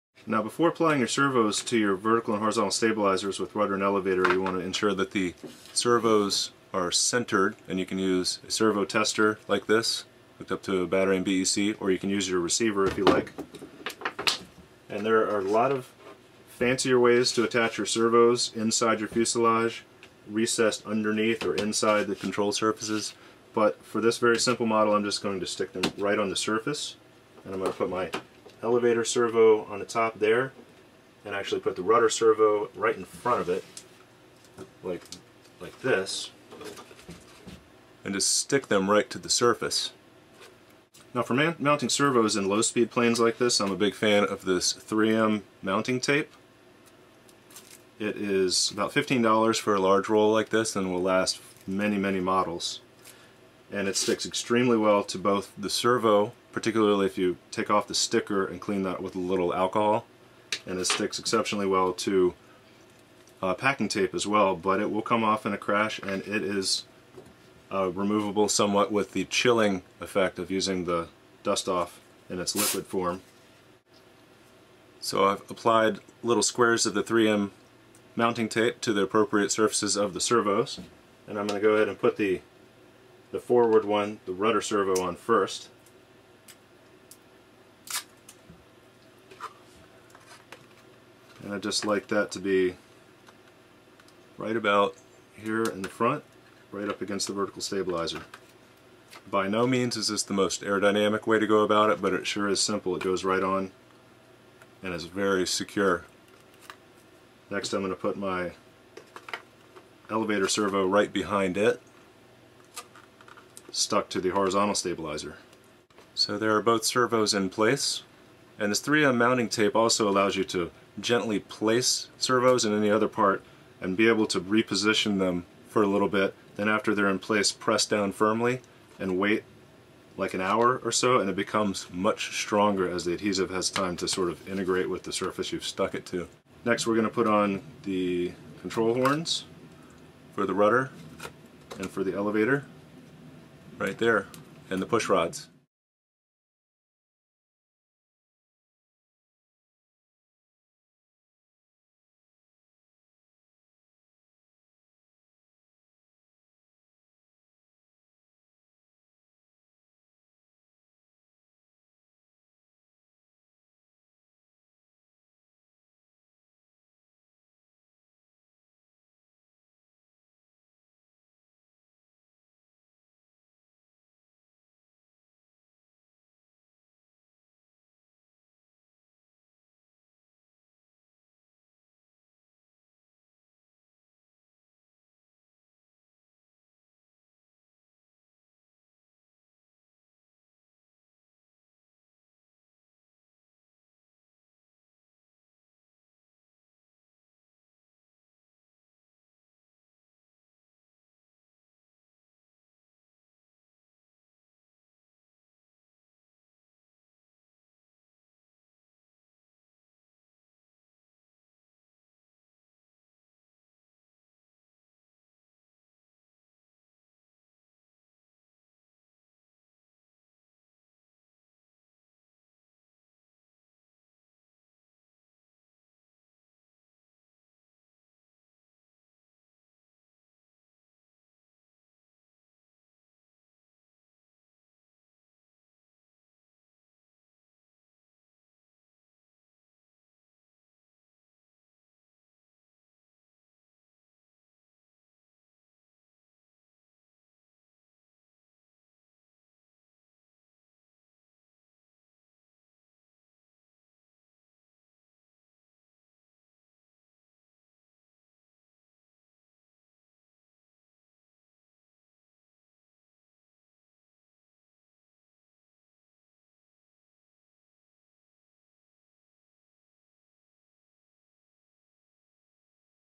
Foamboard Building Techniques: Stabilizers 4 - Servo Installation
Here is a quick, simple, and secure way of affixing your elevator and rudder servos to the tail section using 3M "Scotch" brand heavy-duty mounting tape.  There are more advanced ways of installing servos but this is one idea to get you flying as quickly as possible.